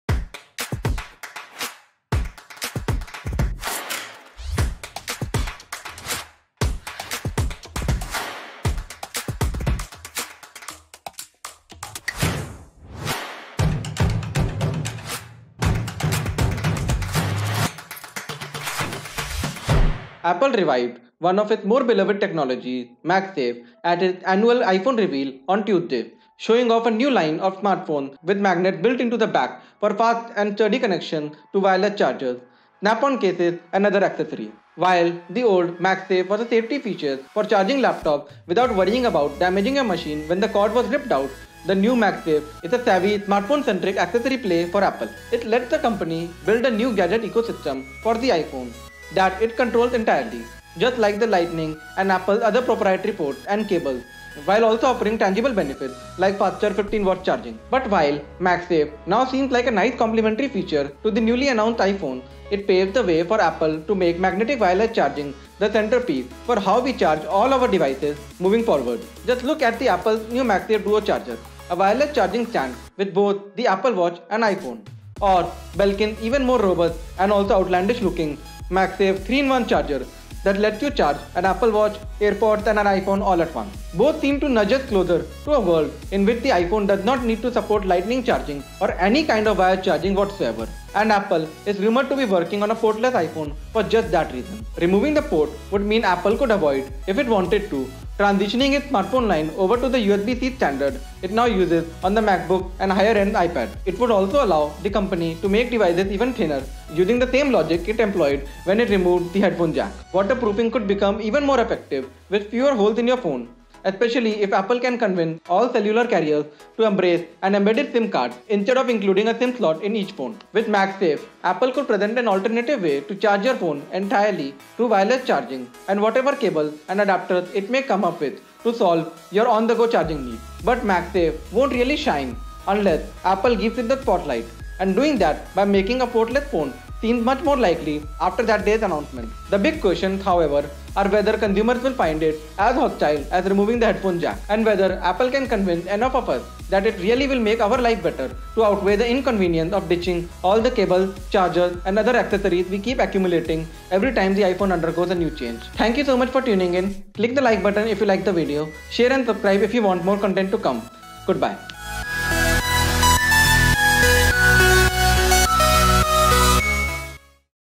Magsafe - leading to a wireless future
Apple, with the announcement of iPhone 12 and 12 Pro, announced Magsafe. Its new way to charge iPhones while using magnets and Qi charging. This brings up the question whether an iPhone without any port at all is close by?